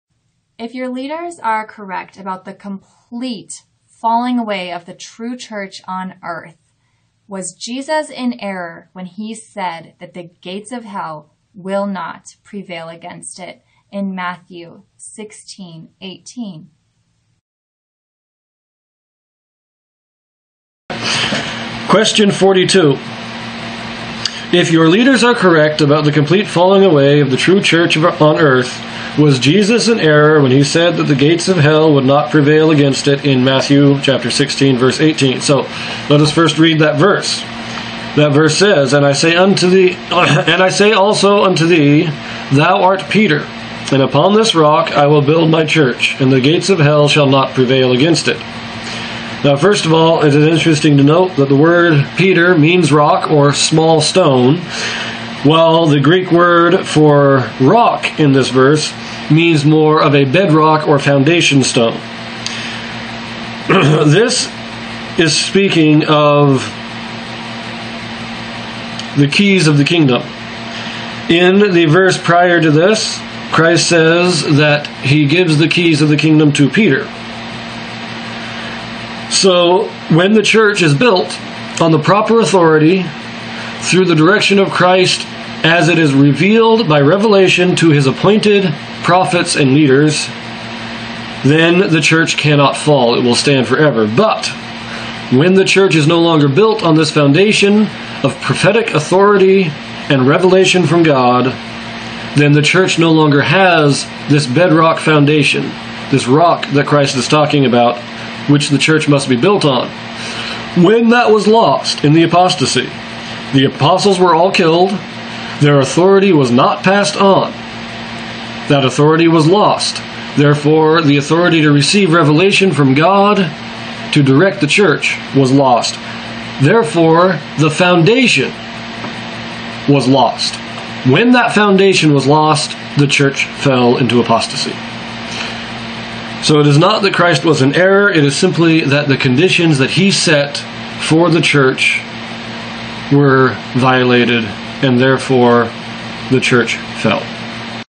Q42: Did the Gates of Hell prevail against the church - an old and only partial answer (LDS answers)
This is not to be taken as an official source of doctrine, as I am not an authorized spokesman for the church. This is just my personal response as a faithful member.